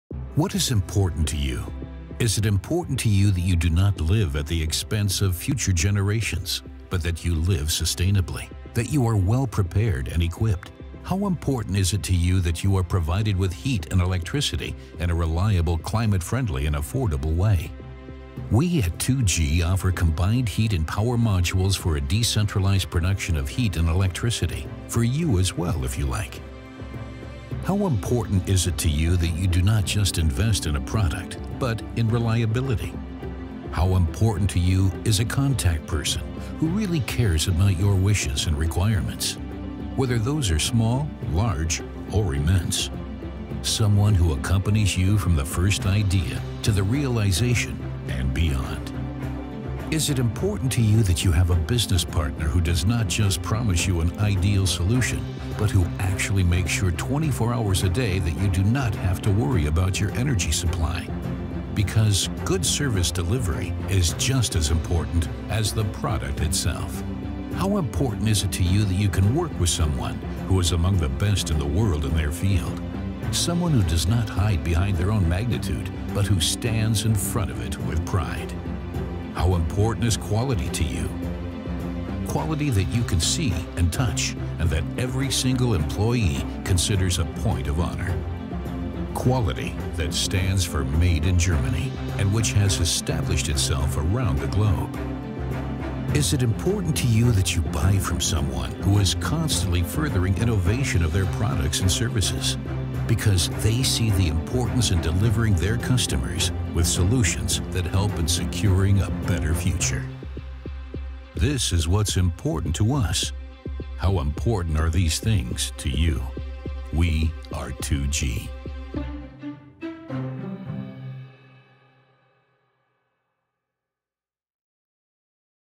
2G Energy Company Video.
What is important to you?
Is it important to you that do not live with the expence of future generations but that you live sustainably?

That you are well prepared and equiped?

How important is it to you that you are provided with heat and electricity in a reliable climate friendly affortable way?
We at 2G offer combined heat and power modules for decentralized production of heat and electricity for you as well if you like.